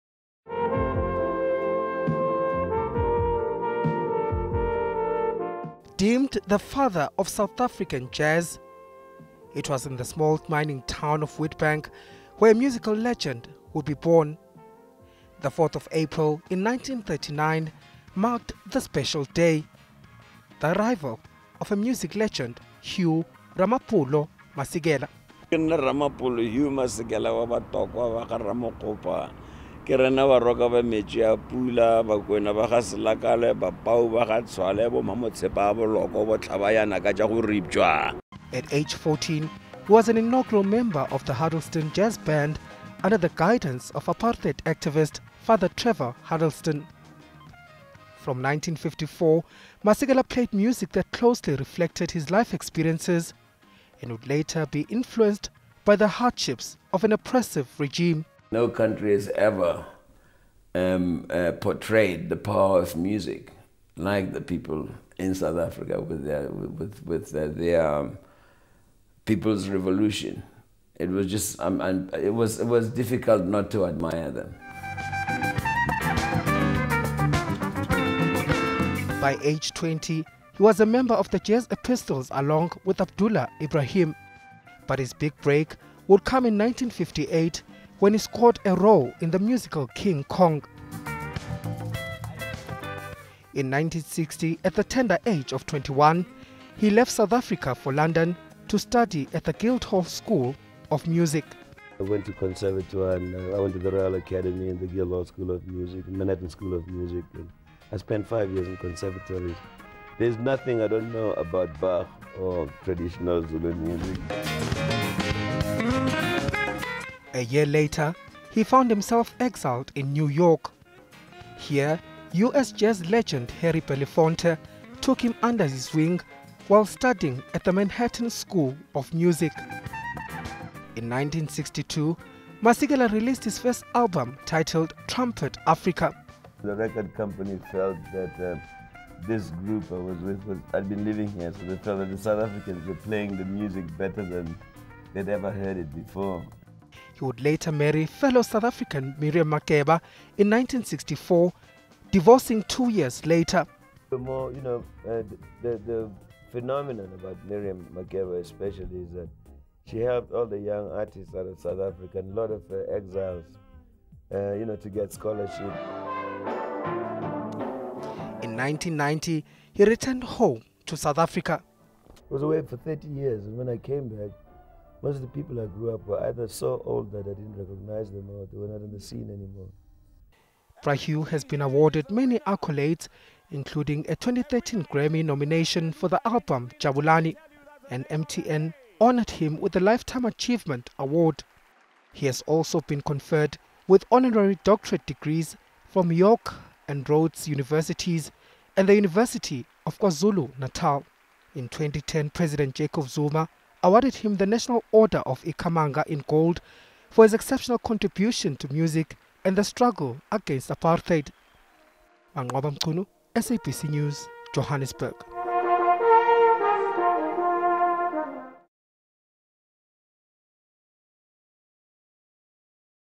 A look at Hugh Masekela's musical career
Hugh Masekela's career spanned more than 50-years, 30 of which were spent in exile. Bra Hugh, as he was affectionately known, used his music to make the world aware of the struggle for liberation in South Africa during the apartheid regime.